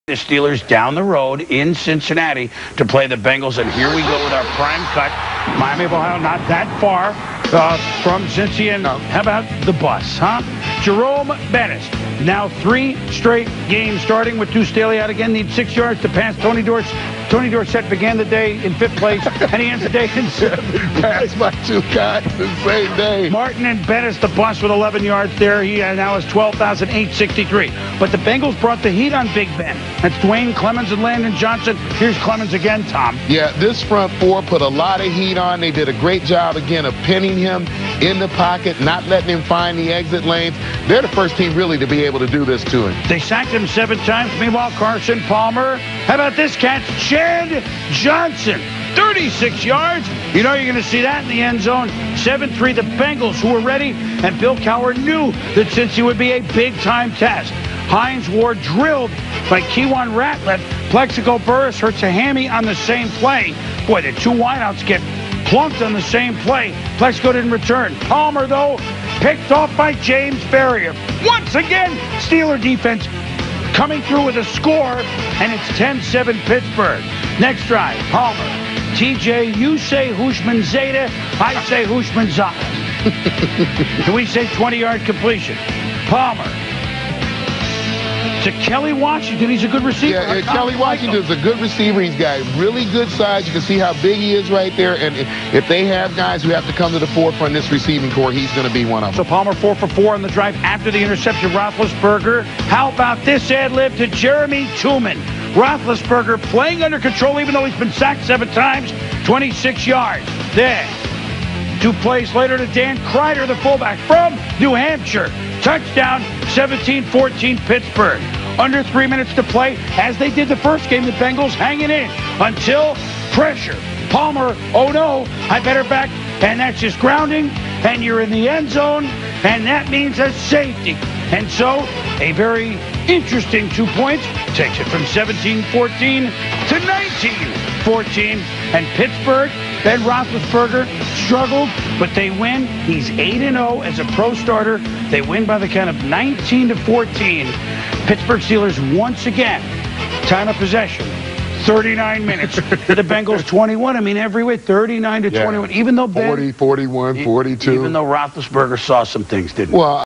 Steelers vs Bengals 2004 Week 11
Game Time: 1:03 p.m. EST
Weather: 50 degrees, relative humidity 80%, wind 7 mph, wind chill 0
Game Attendance: 65,780
Referee: Larry Nemmers